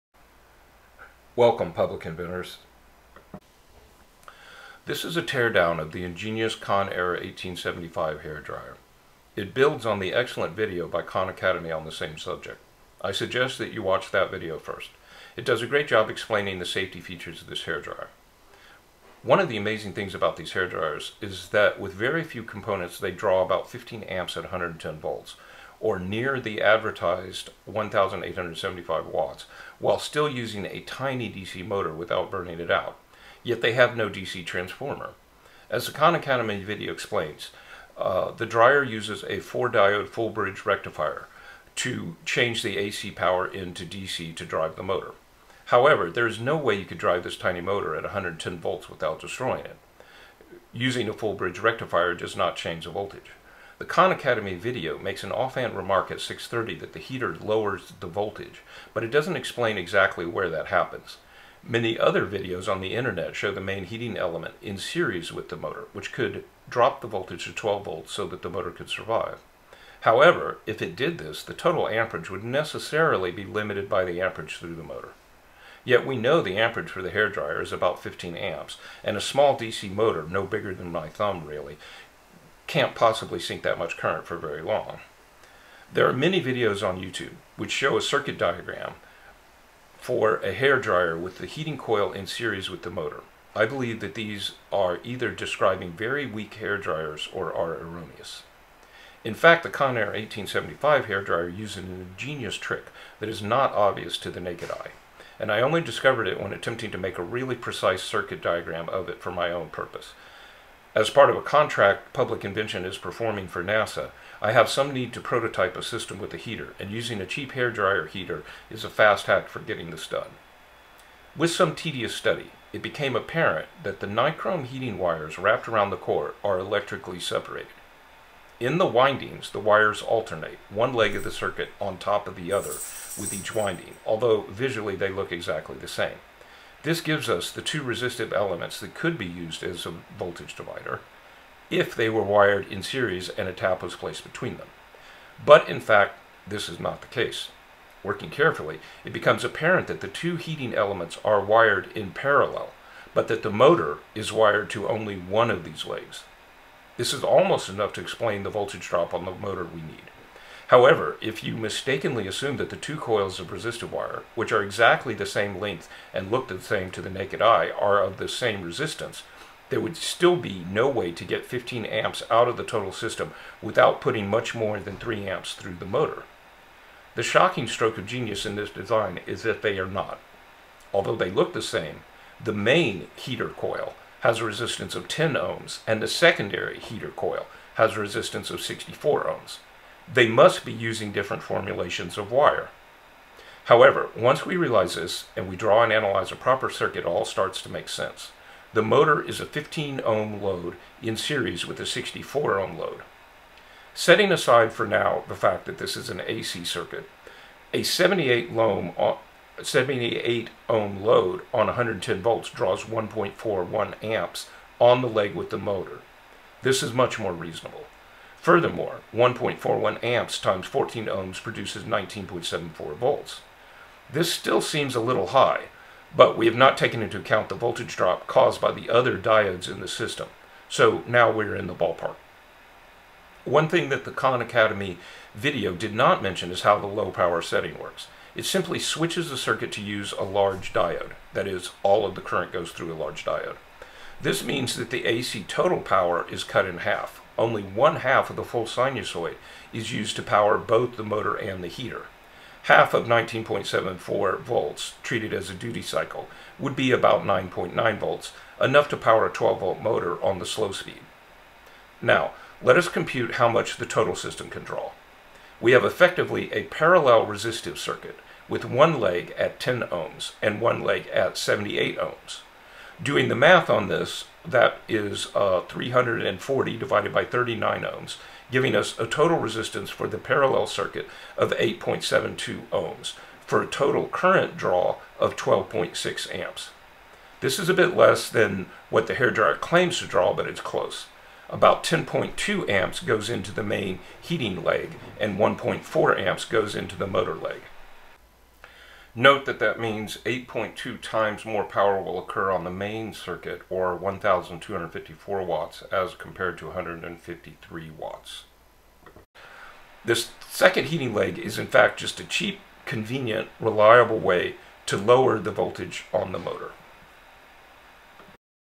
Teardown of the ConAir 1875 HairDryer Circuit
This is a response by Khan Academy. I took apart a modern hair dryer and discovered an ingenious design which seems to be misrepresented in many videos about the electronic circuit for them.